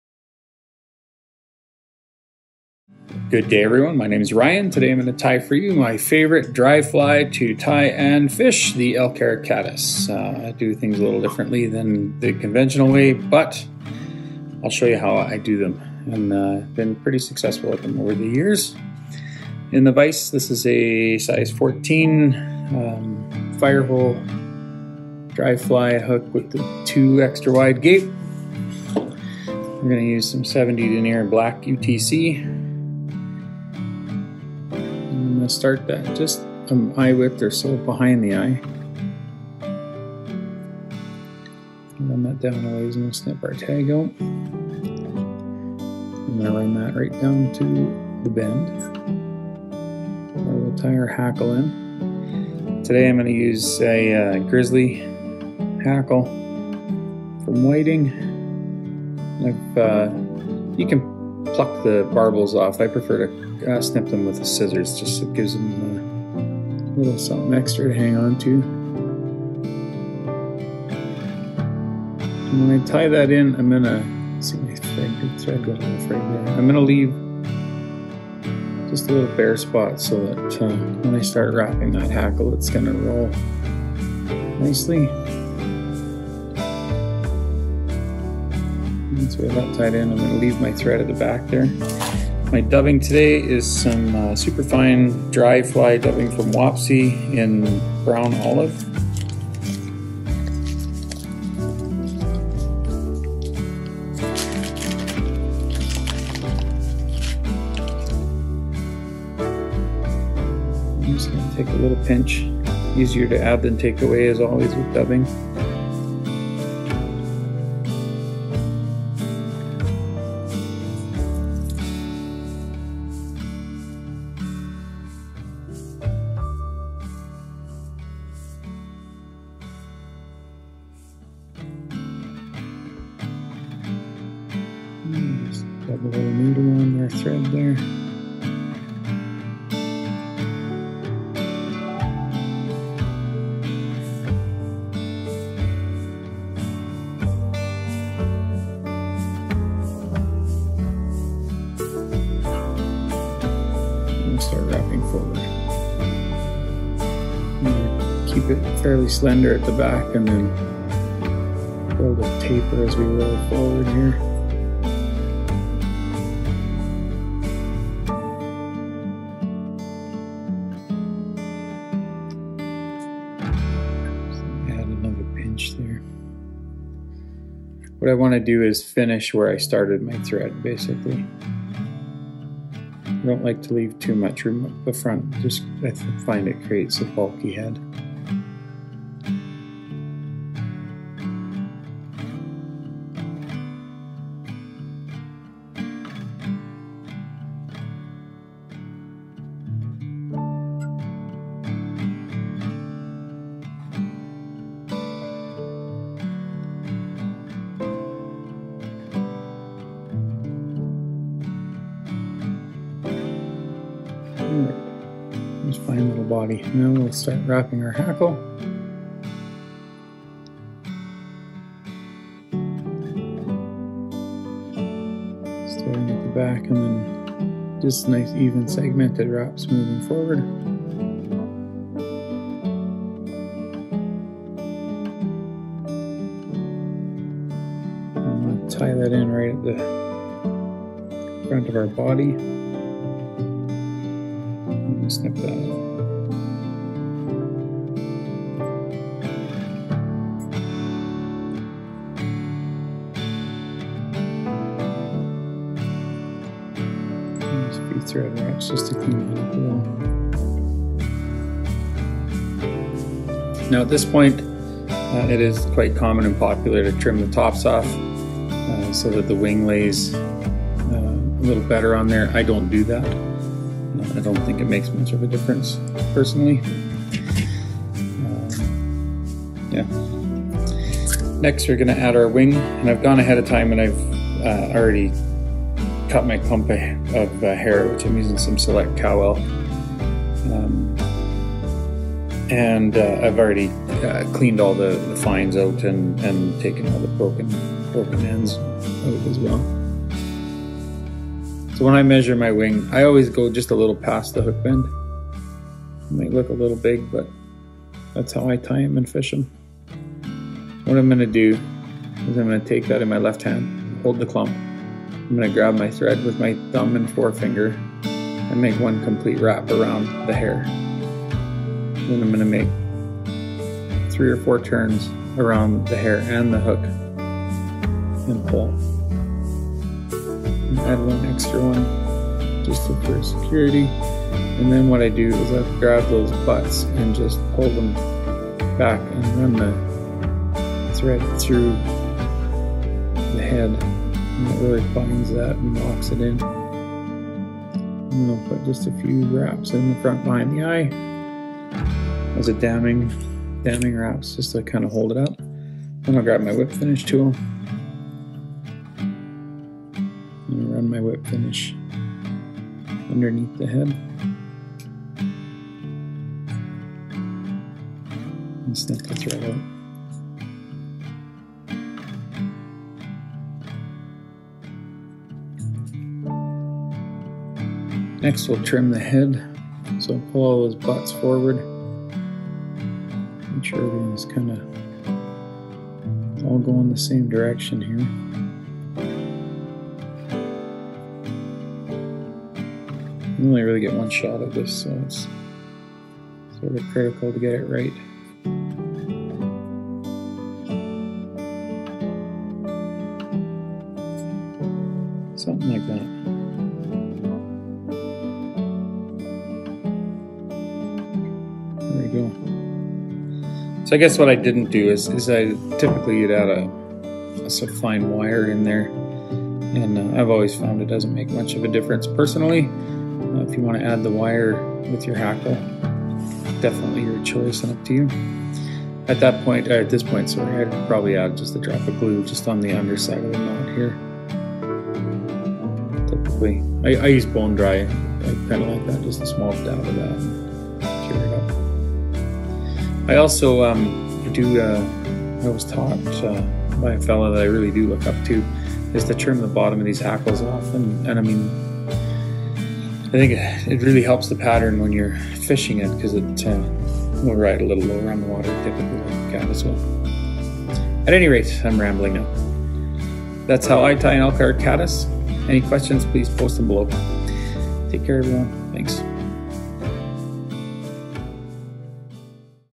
Ryan Storch tying an Elk Hair Caddis Fly
Gather the materials from the list below to tie this Elk Hair Caddis Fly along with Norvise Team Member, Ryan Storch. Please remember to tag Norvise on Facebook or Instagram and let us know if you gave this pattern a try.

ELK HAIR CADDIS MATERIALS LIST:
- Dry Fly Hook - Size 20 to 8
- 70d UTC in Black
- Gizzy Hackle
- Superfine Dry Fly Dubbin in Brown-Olive
- Bleached Elk Hair